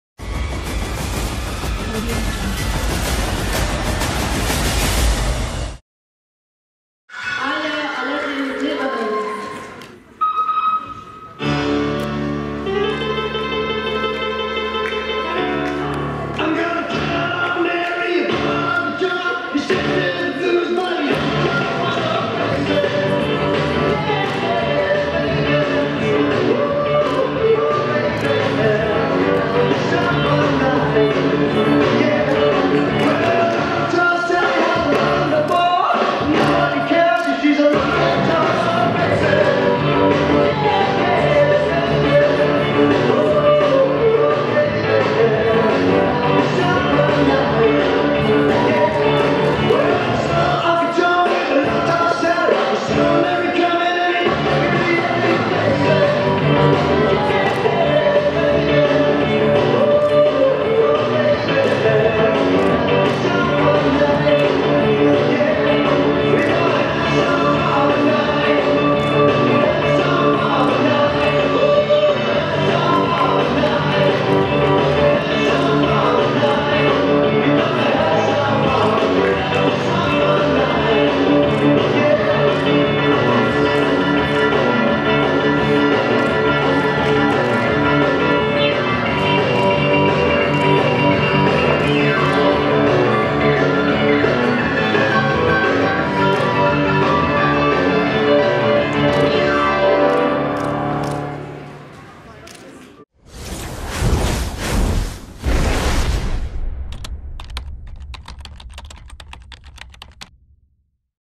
Aya Alaeddin Clubs
6th edition of International Rhythmic Gymnastics Competition LUXEMBOURG CUP 2017, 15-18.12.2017, Luxembourg, Belair
Aya Alaeddin LBN